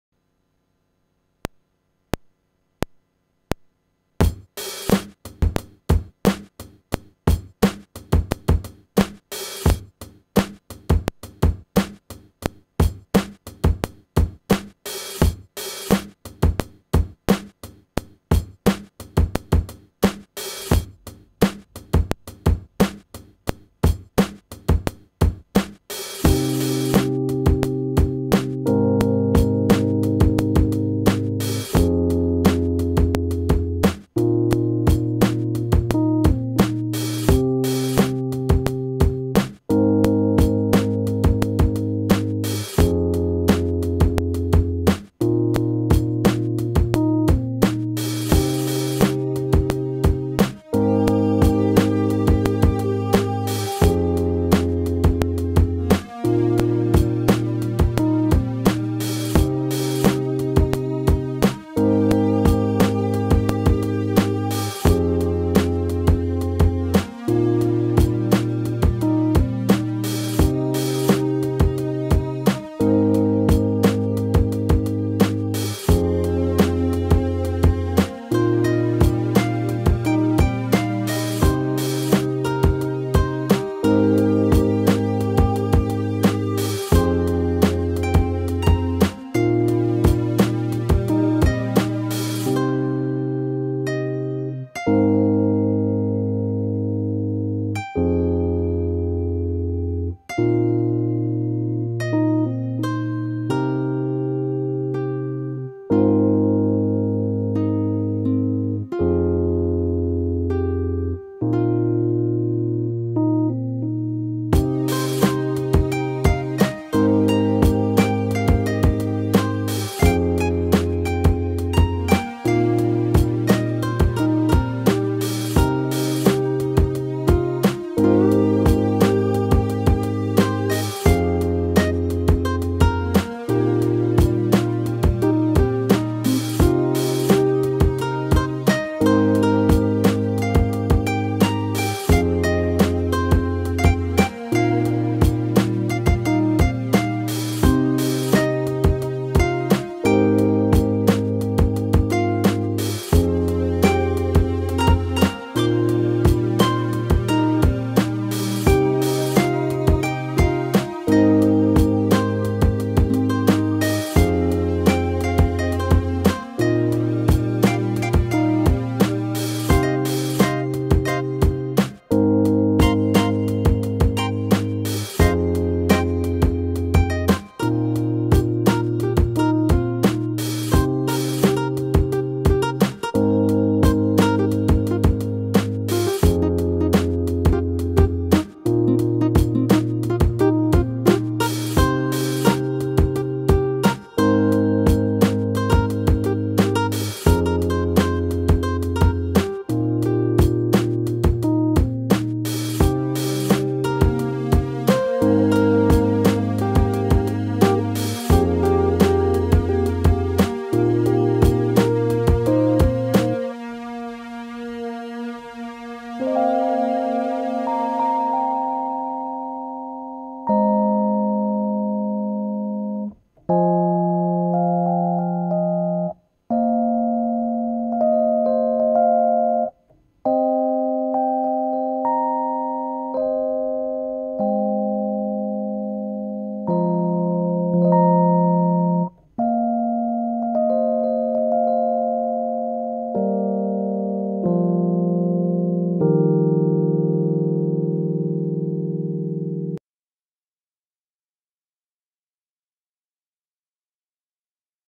Keys
It's been a while since I posted a video. This is just practicing with some rhodes sounds and other stuff.